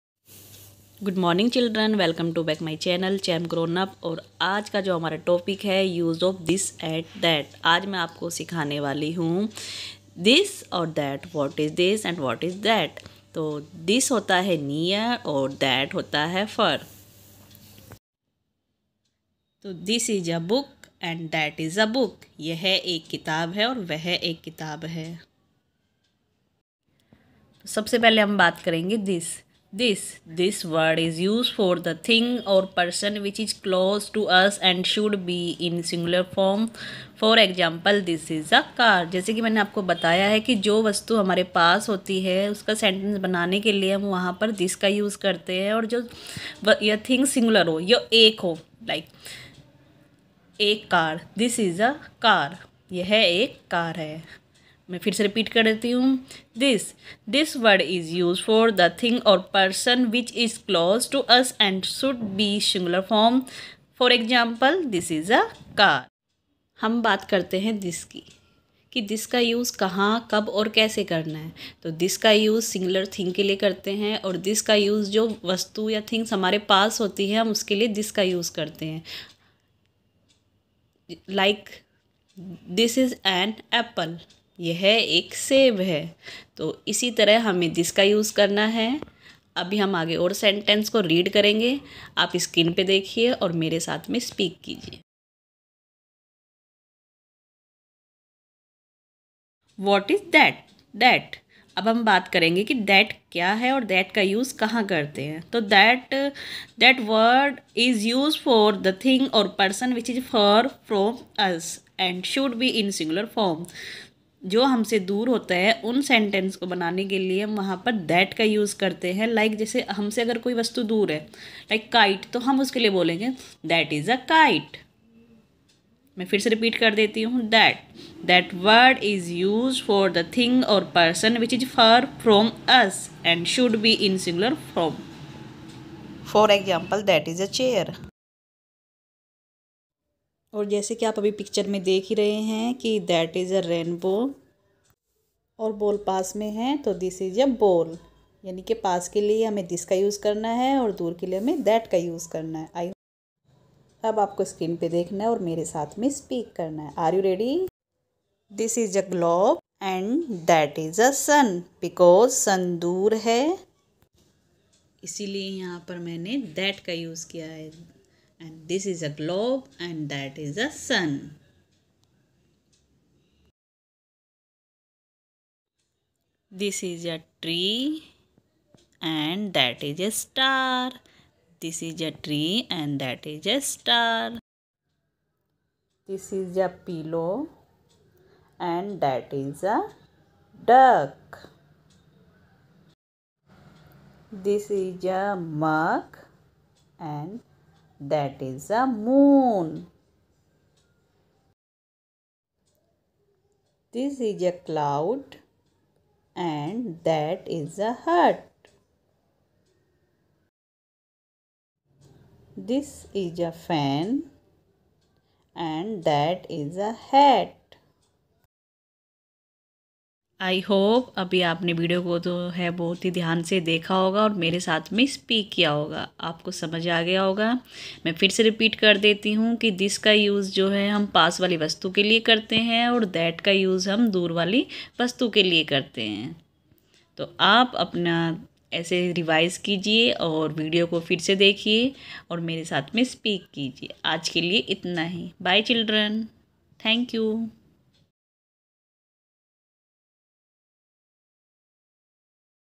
Let's learn use of this and that
apne bacho ko easy way m this and that kavuse sikhaye